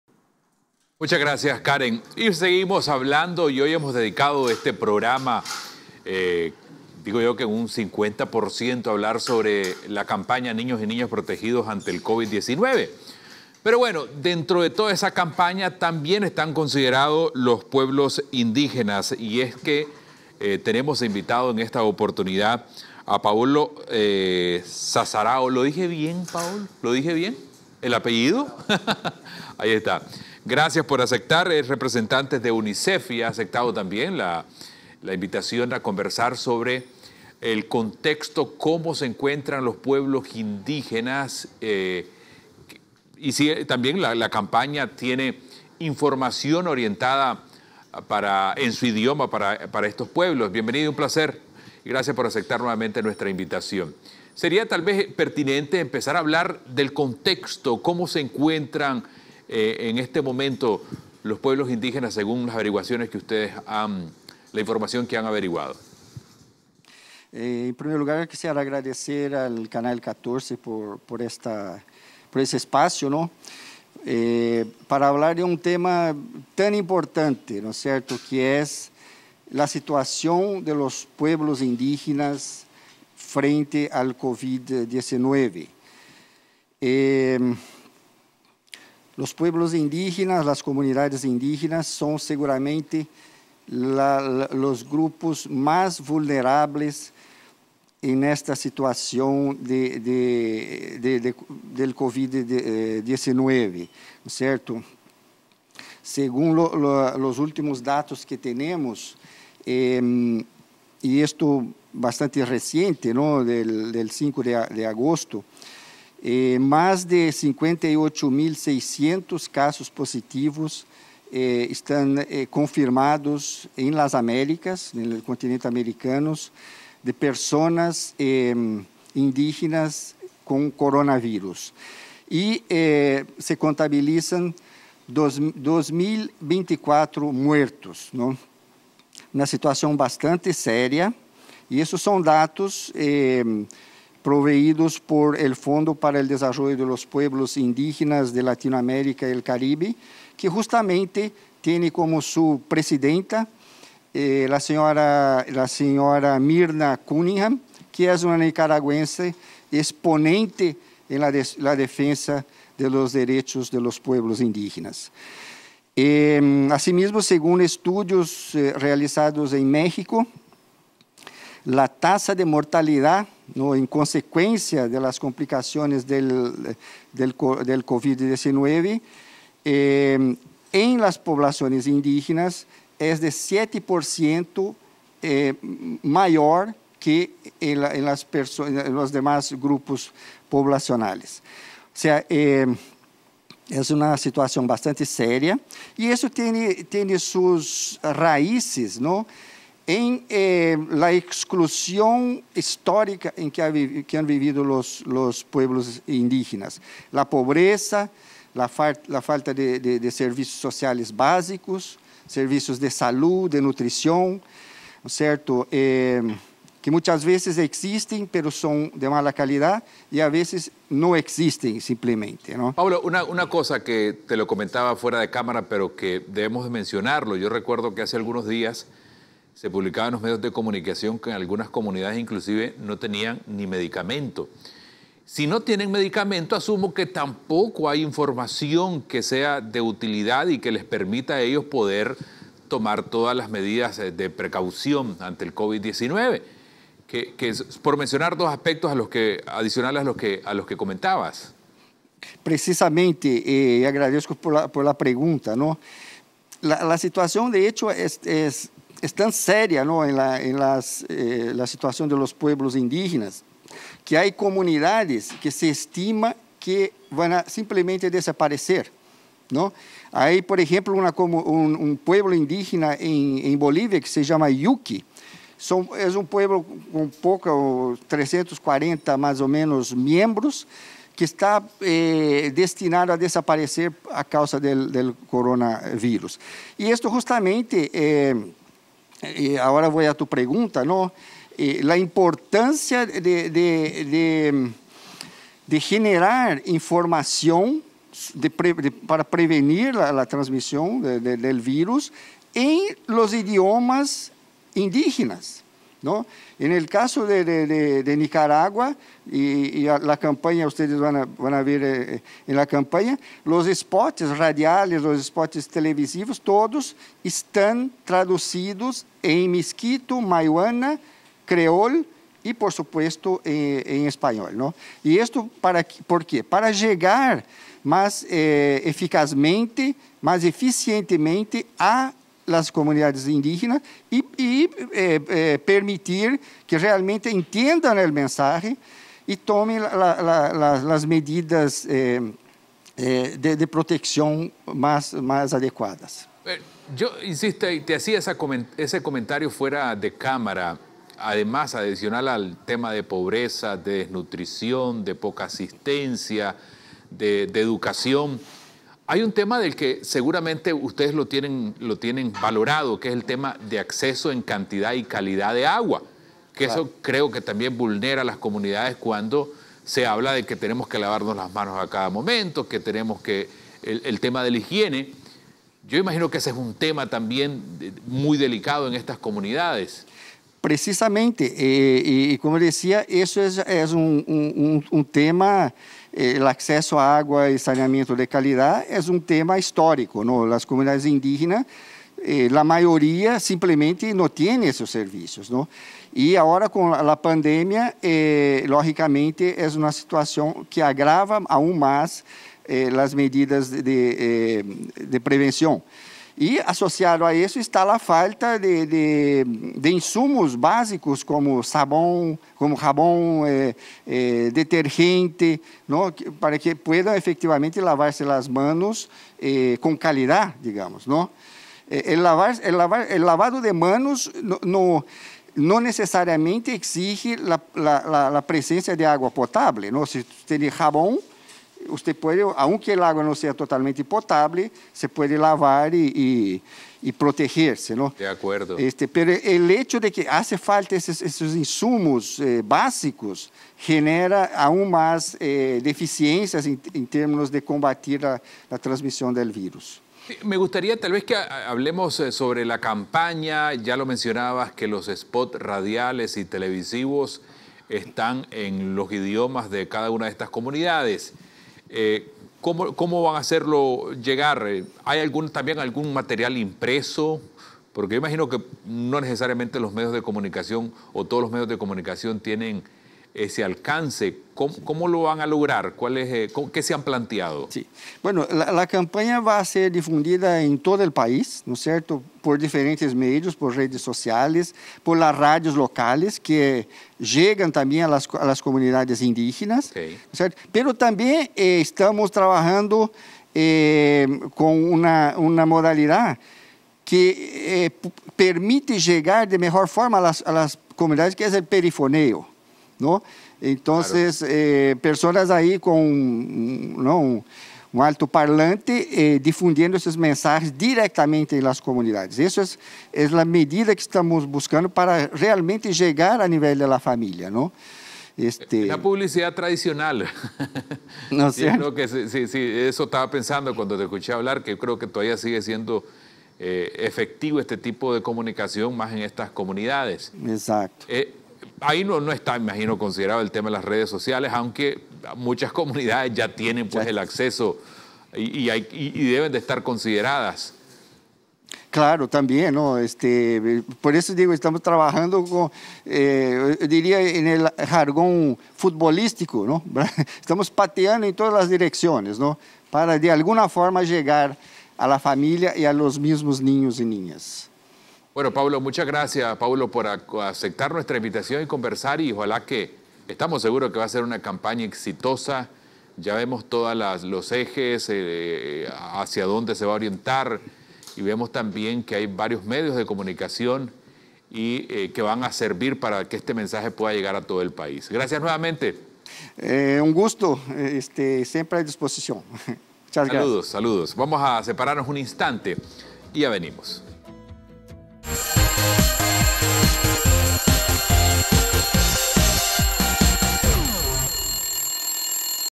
La importancia de proteger a los pueblos indígenas ante el Covid-19
¿Cómo se encuentran los pueblos indígenas ante la amenaza del Covid-19? Paulo Saddarao, representante de la UNICEF y parte de la campaña "Niñas y niños protegidos ante el Covid-19" nos comenta el contexto de los pueblos ante la pandemia.